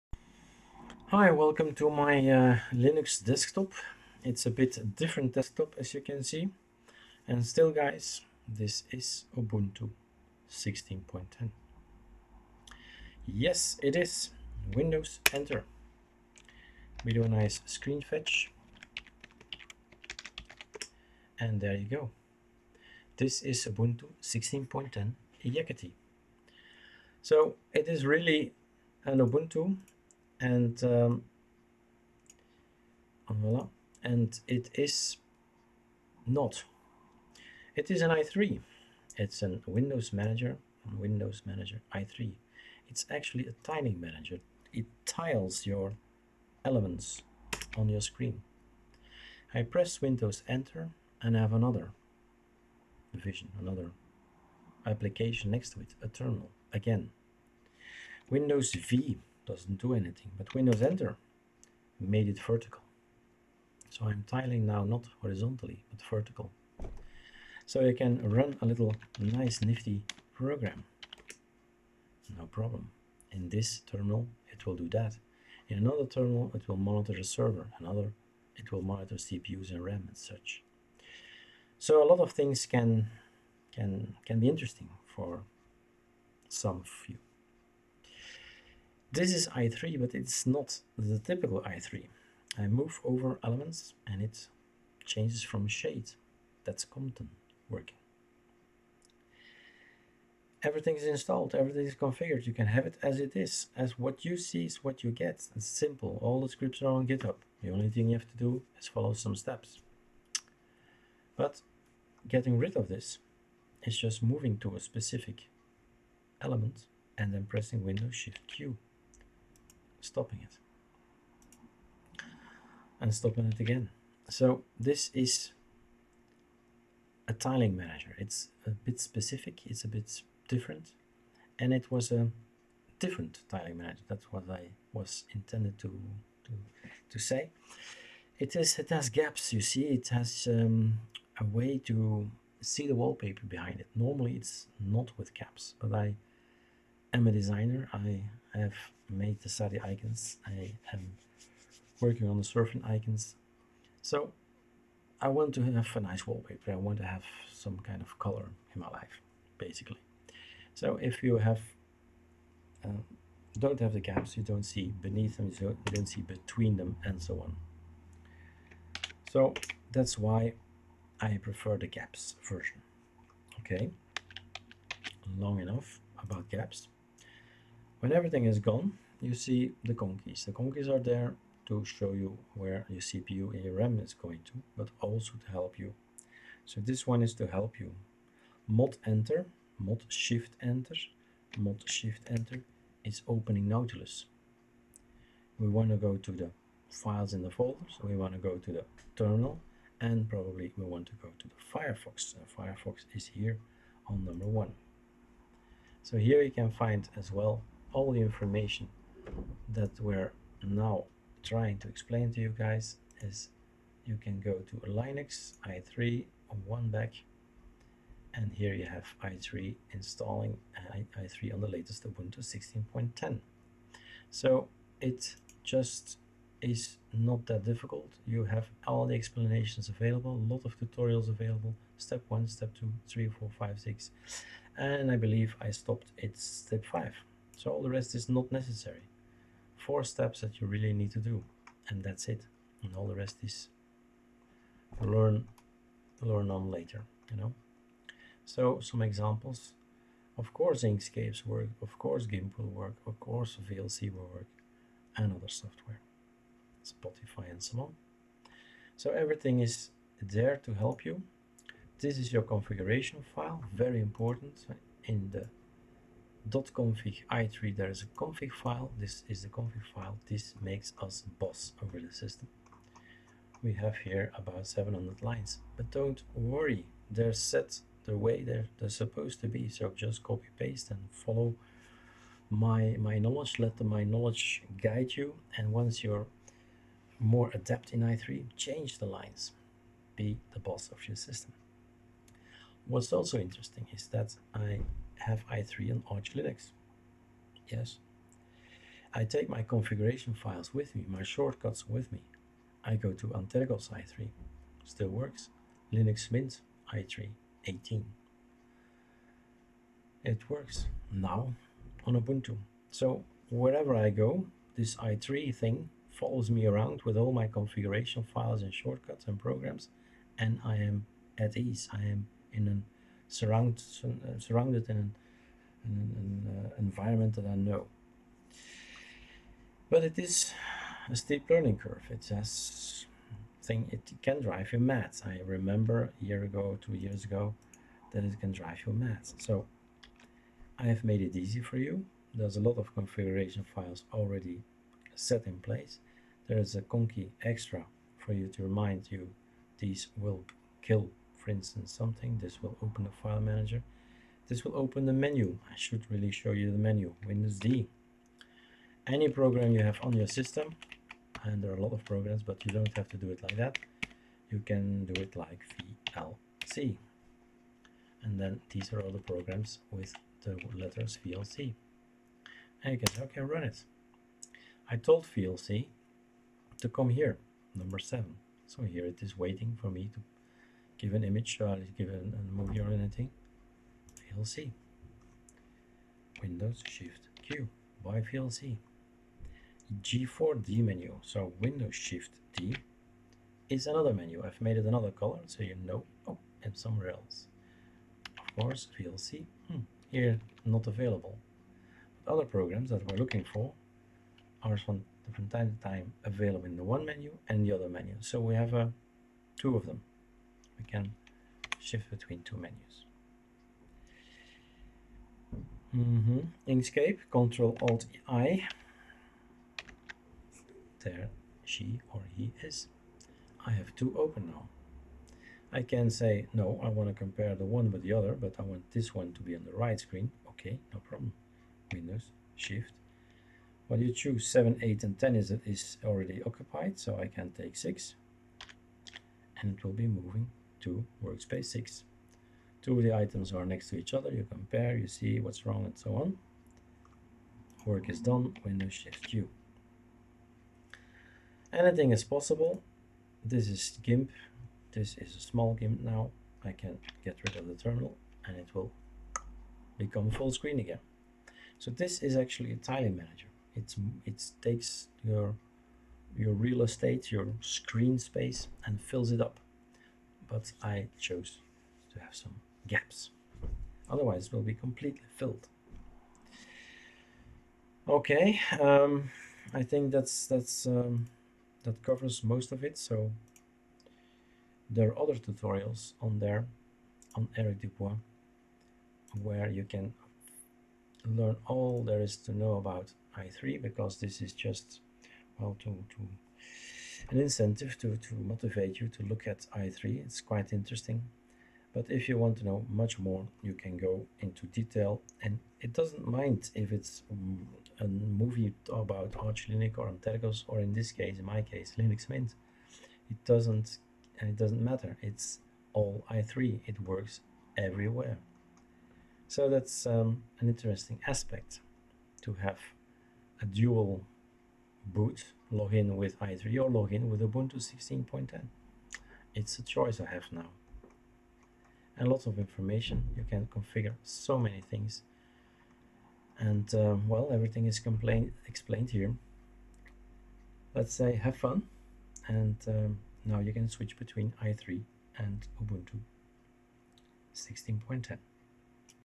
Installing i3 on Ubuntu 16.10
Easy installation of i3 wm tiling manager on Ubuntu 16.10.